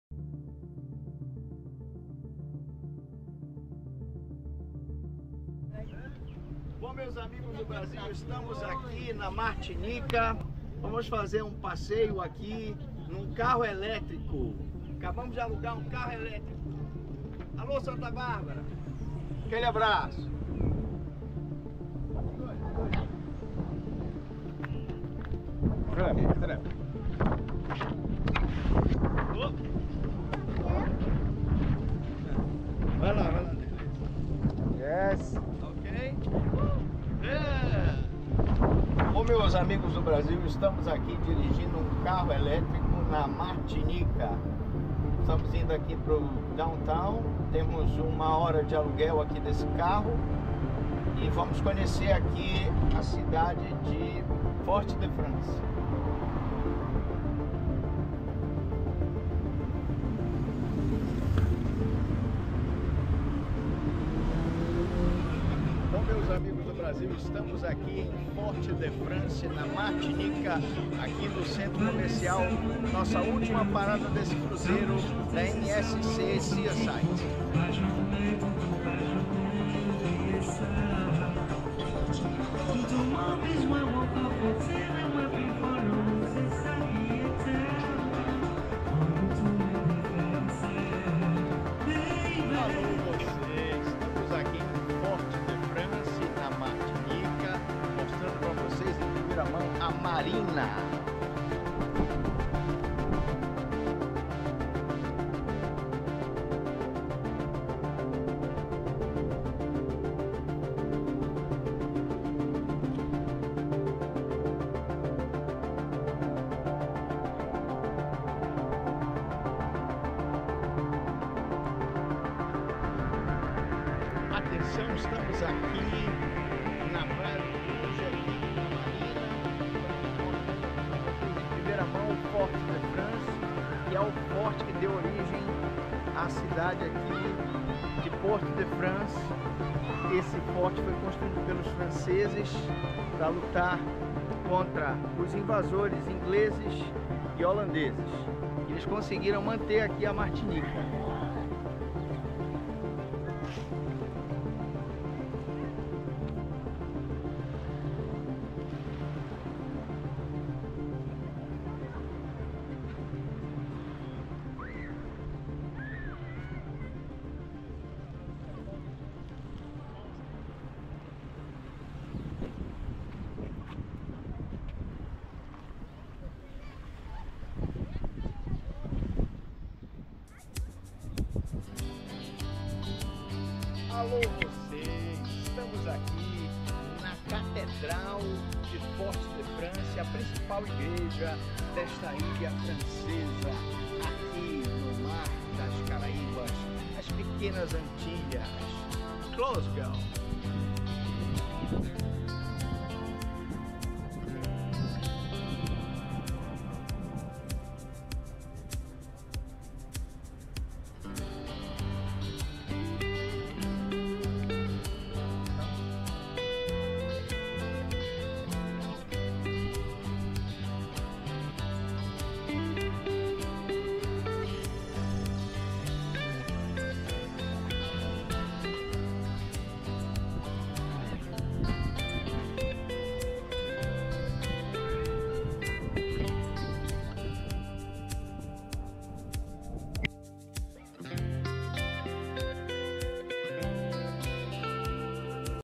08. Martinica 2024
Forte da França ou Forte de França é a cidade capital da Martinica, ilha do Caribe que é departamento de ultramar da França. É uma das maiores cidades caribenhas. Suas principais exportações incluem açúcar, rum, frutas enlatadas e cacau